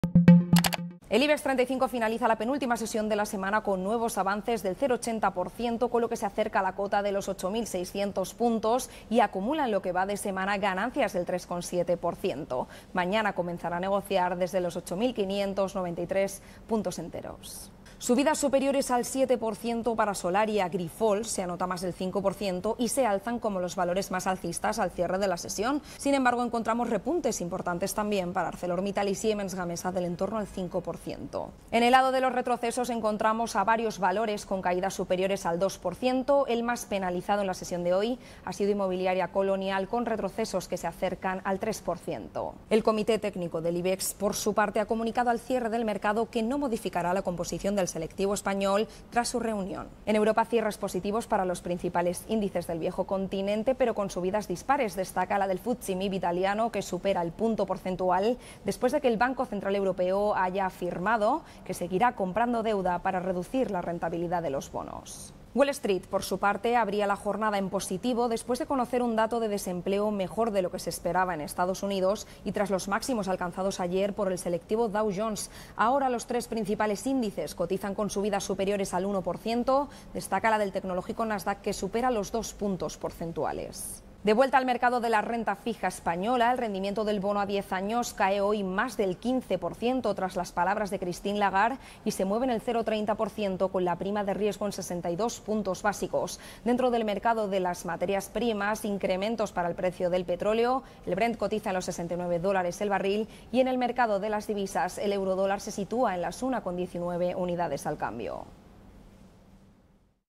El Ibex 35 suma una jornada más de avances y roza los 6.900 puntos
Madrid, 11 mar (EFE).- El Ibex 35 finaliza la sesión del jueves con nuevos avances del 0,80 % con los que se acerca a los 8.600 puntos y mantiene ganancias semanales del 3,7 %.